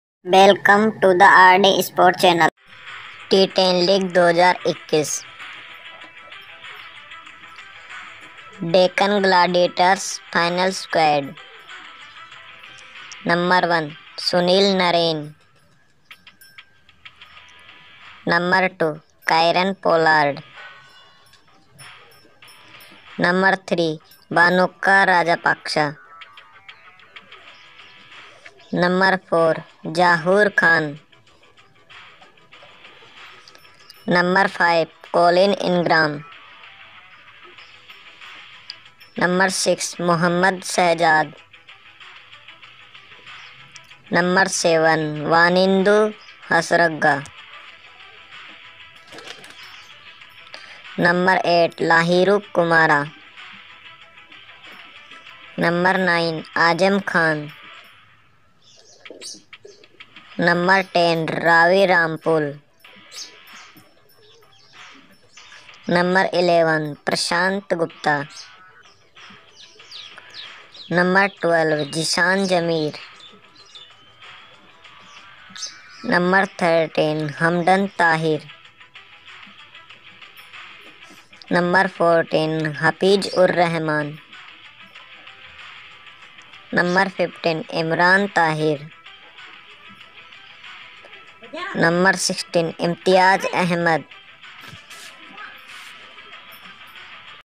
Deccan Gladiators Final Squad T10 League 2021 | T10 League 2021 Deccan Gladiators Full & Final Squad
Welcome to  RD Sports

Here is the official Squad :

Sunil Narine (icon)
Bhanuka Rajapaksa
Zahoor Khan
Colin Ingram
Mohammad Shahzad
Wanindu Hasaranga
Lahiru Kumara
Azam Khan
Ravi Rampaul
Prashant Gupta
Zeeshan Zameer
Hamdan Tahir
Hafeez-ur-Rehman
Imran Tahir
Imtiyaz Ahmed